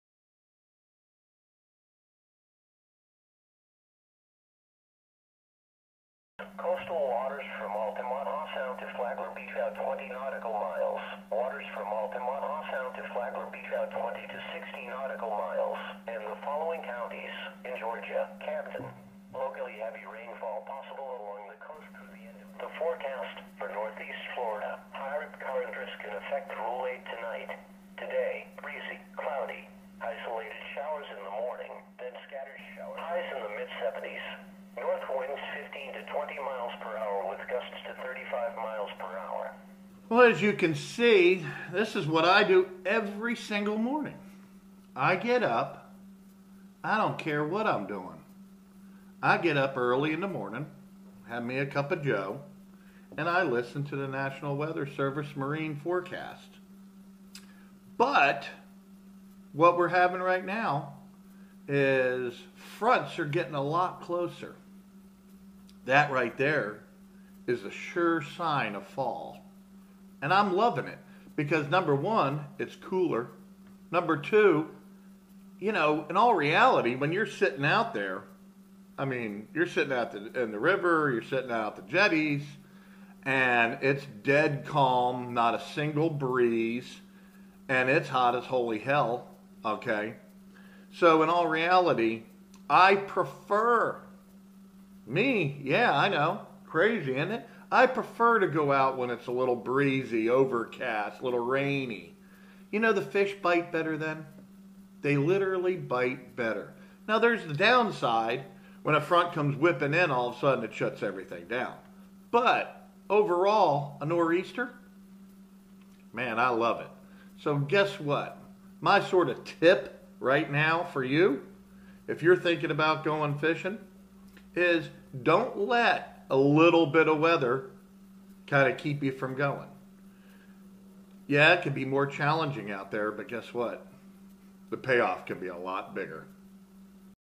MARINE FORECAST: Fishing Guide's tip:
"JETTYWOLF LIVE"

“Don’t’ head out on the water without checking the weather first! Captain Dave walks you through what to listen for on the National Weather Service’s marine forecast and talks about why he loves fishing in Fall weather. It may be challenging out on the water, but don’t let that hold you back. The pay-off can be A LOT better!”